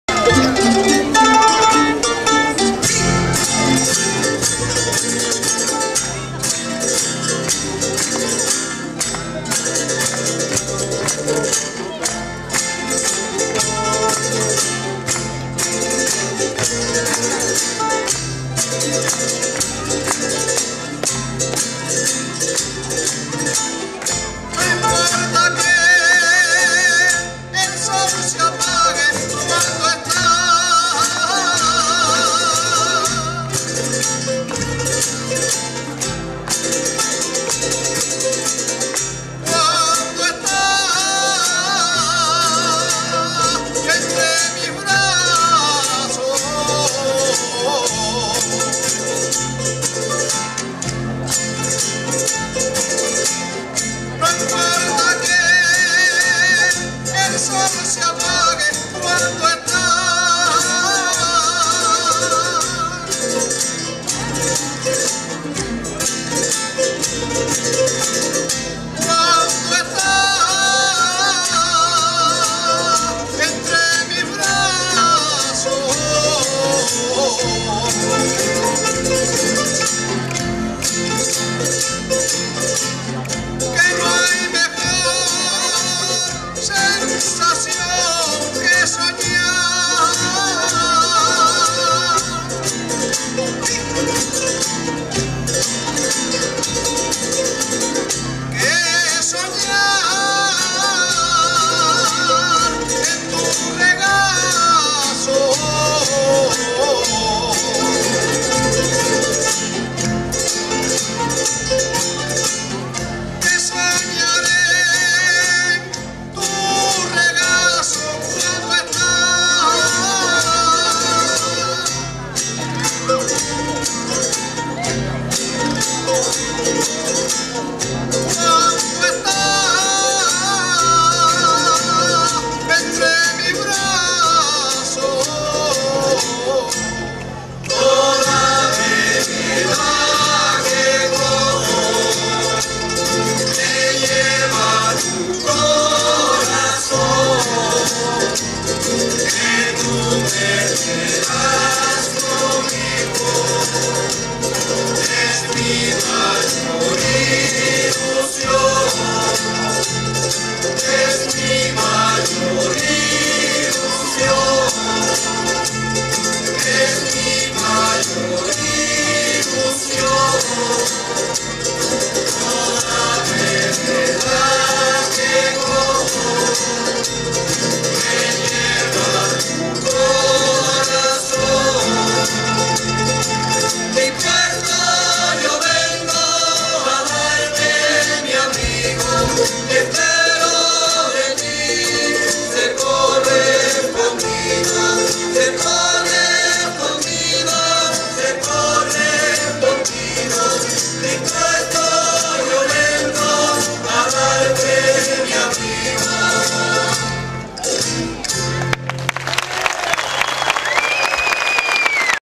Agrupación Músico Cultural Timbayba, Lanzarote
Agrupación Músico Cultural Timbayba, Lanzarote, na V Semana Europeia de Folclore, 2 a 6 de Setembro 2008. Jardim Municipal, Funchal, Madeira